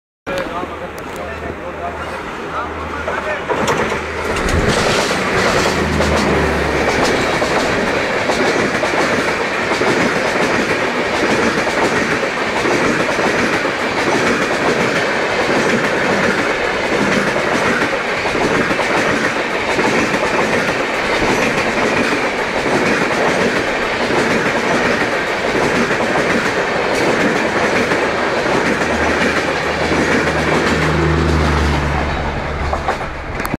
IRFCA- 12951 Mumbai-Delhi Rajdhani at Bandra
12951 Mumbai Central - NDLS Rajdhani entering the 60 KMPH PSR becoz of the S-shape curve created on the main line tracks due to the Harbour line Rail-over-Bridge at Bandra outer in Mumbai Suburban Section.

Date: 24/04/2011
Time: 04:54pm